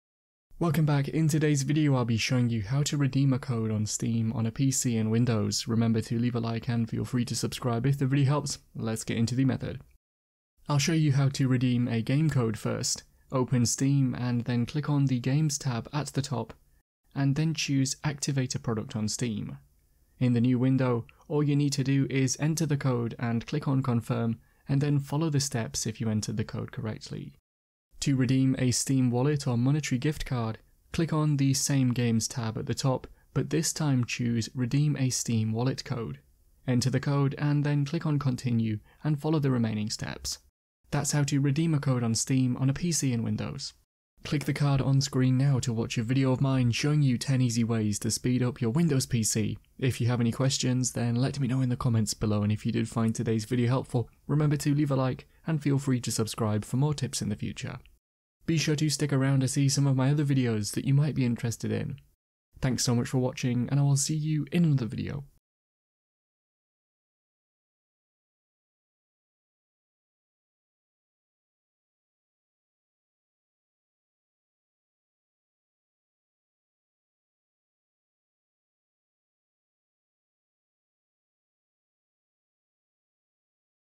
How to Redeem a Code on Steam on PC
Want to know how to redeem a code on Steam on PC in Windows 10 or Windows 11? This video will show you how to redeem Steam code in Windows 11 or Windows 10 on your PC or laptop. You might want to know how to redeem game code on steam on PC in Windows 10 or 11 to if you received a code as a gift. Hopefully today’s video helps show you how to redeem Steam game code on PC or laptop in Windows 11 or Windows 10!